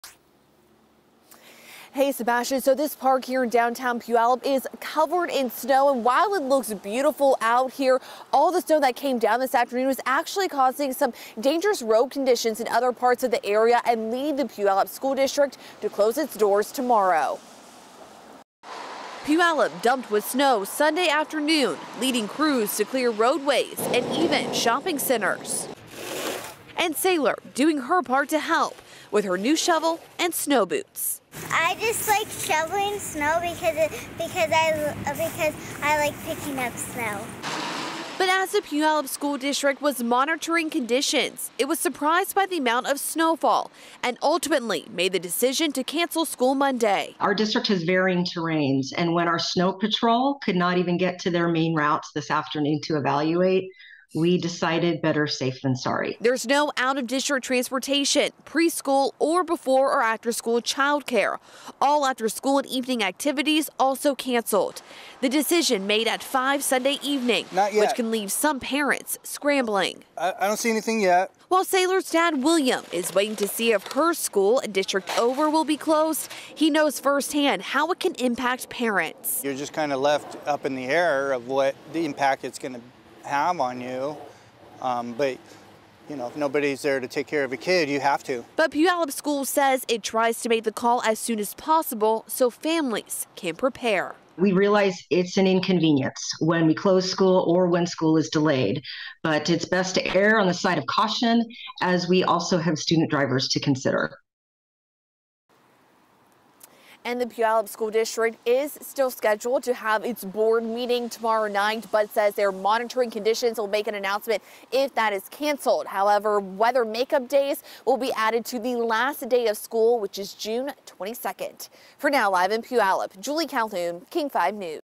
Puyallup School District closes Monday after heavy snowfall
Puyallup School District will not be open Monday after a snowy weekend.